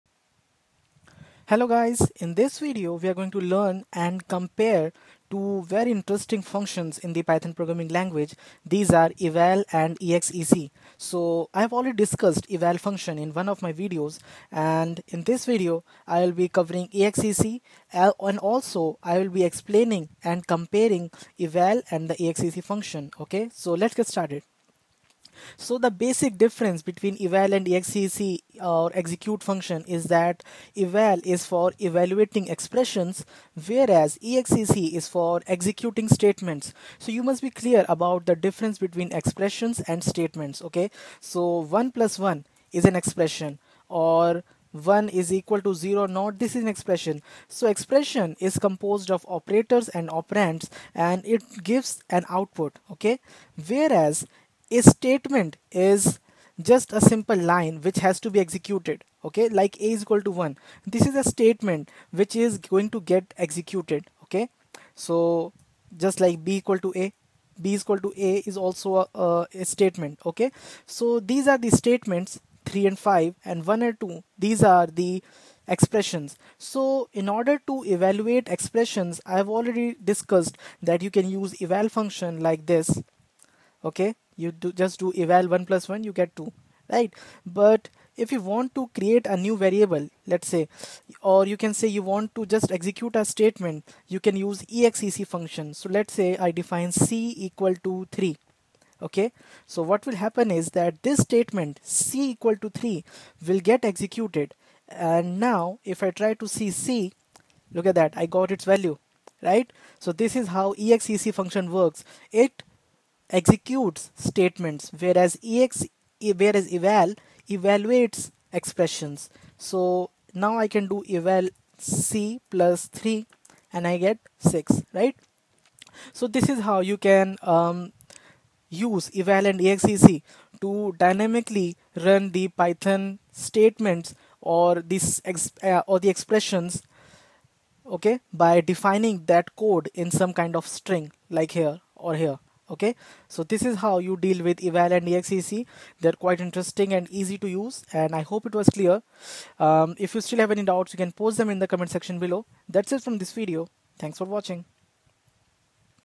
eval vs exec in Python
This video explains the concept of eval and exec function in python and compares them.